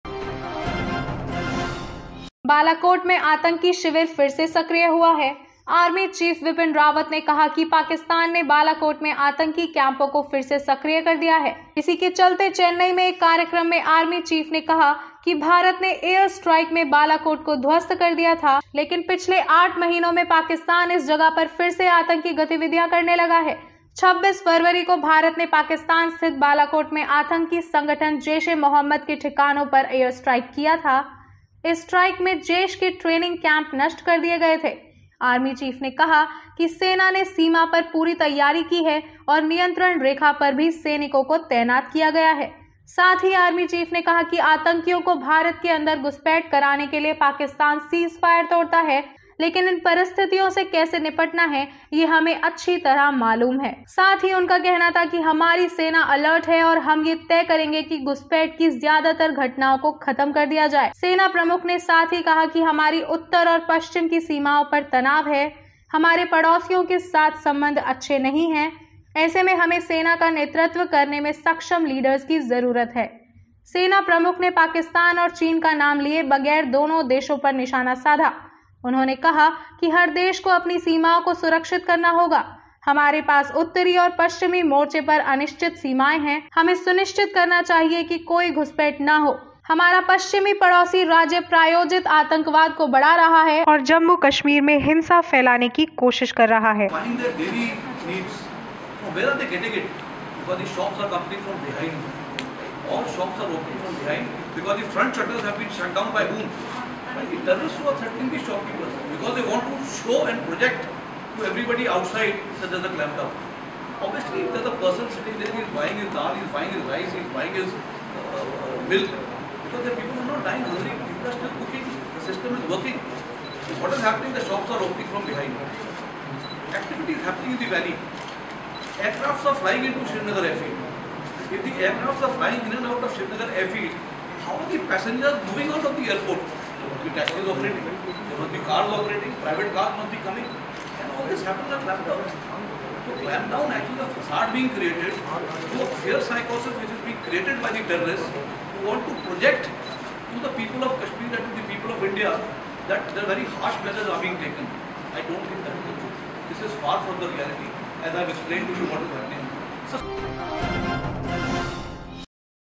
Balakot में आंतकी हुए Active ! Army Chief Bipin Rawat ! बालाकोट में फिर से सक्रिय हुआ आतंकी शिविर !
बालाकोट में आतंकी शिविर फिर से एक्टिव हुआ है , आर्मी चीफ बिपिन रावत ने कहा है कि पाकिस्तान ने बालाकोट में आतंकी कैंपों को फिर से सक्रिय कर दिया है. इसी के चलते चेन्नई में एक कार्यक्रम में आर्मी चीफ ने कहा कि भारत के एयरस्ट्राइक में बालाकोट को ध्वस्त कर दिया गया था. लेकिन पिछले आठ महीनों में पाकिस्तान इस जगह पर फिर से आतंकी गतिविधियां करने लगा है.  26 फरवरी को भारत ने पाकिस्तान स्थित बालाकोट में आतंकी संगठन जैश-ए-मोहम्मद के ठिकानों पर एयरस्ट्राइक किया था. इस स्ट्राइक में जैश के ट्रेनिंग कैंप नष्ट कर दिए गए थे. आर्मी चीफ ने कहा कि सेना ने सीमा पर पूरी तैयारी की है और नियंत्रण रेखा पर भी सैनिकों को तैनात किया गया है. साथ ही आर्मी चीफ ने कहा कि आतंकियों को भारत के अंदर घुसपैठ कराने के लिए पाकिस्तान सीजफायर तोड़ता है, लेकिन इन परिस्थितियों से कैसे निपटना है, ये हमें अछि तरह मालूम है.

The terrorist camp has been activated again in Balakot, Army Chief Bipin Rawat has said that Pakistan has reactivated the terrorist camps in Balakot. Due to this, in an event in Chennai, the Army Chief said that Balakot was demolished in India's Airstrike. But in the last eight months, Pakistan has started doing terrorist activities again at this place. On 26 February, India airstrike at the bases of terrorist organization Jaish-e-Mohammed in Balakot, Pakistan. Jaish's training camps were destroyed in this strike. The Army Chief said that the army has made full preparations on the border and troops have also been deployed along the Line of Control. At the same time, the Army Chief said that Pakistan breaks a ceasefire to infiltrate terrorists inside India, but we know how to deal with these situations.